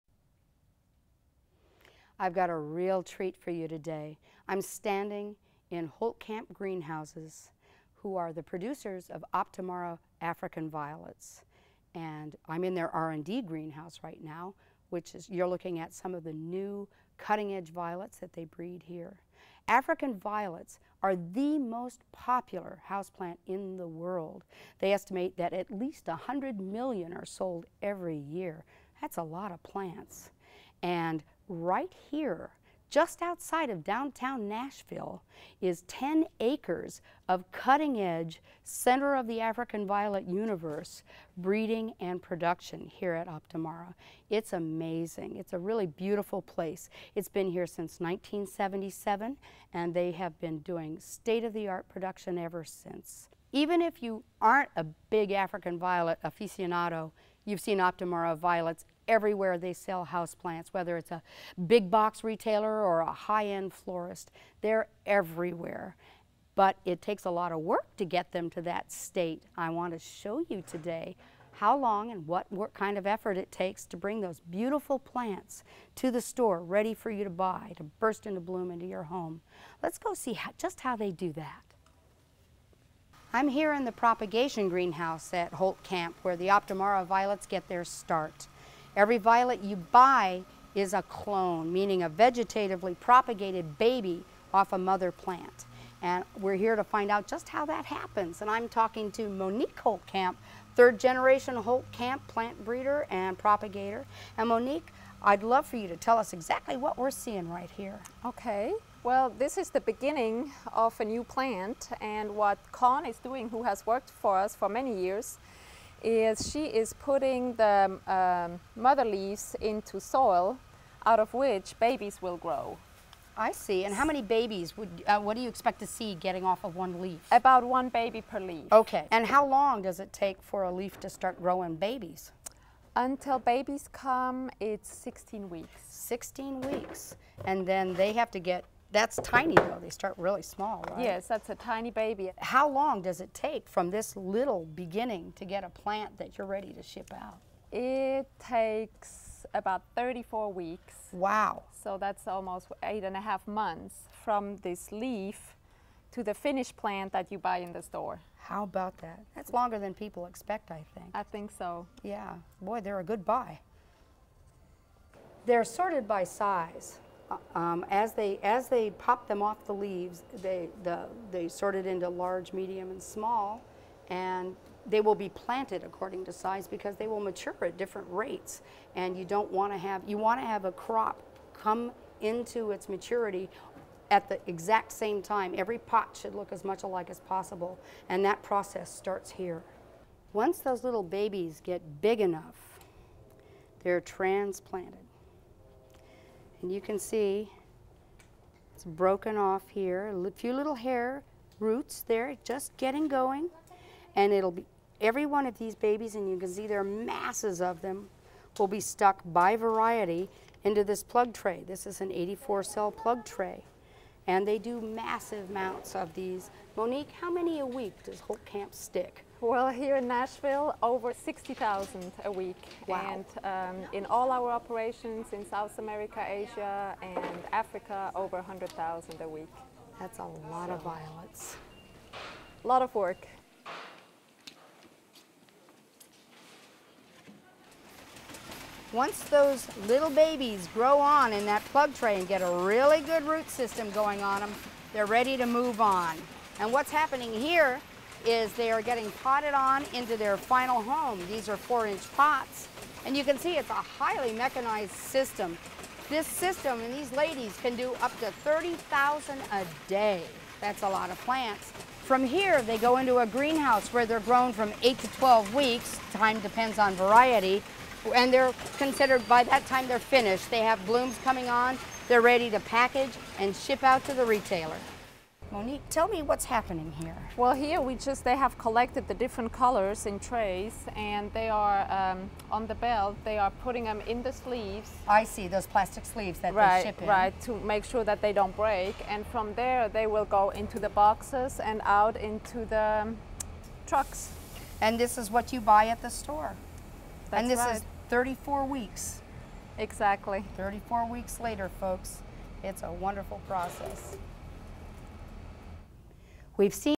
Optimara Nashville African Violet Production Facility
This video features the production of the Optimara African Violet at the Nashville Production facility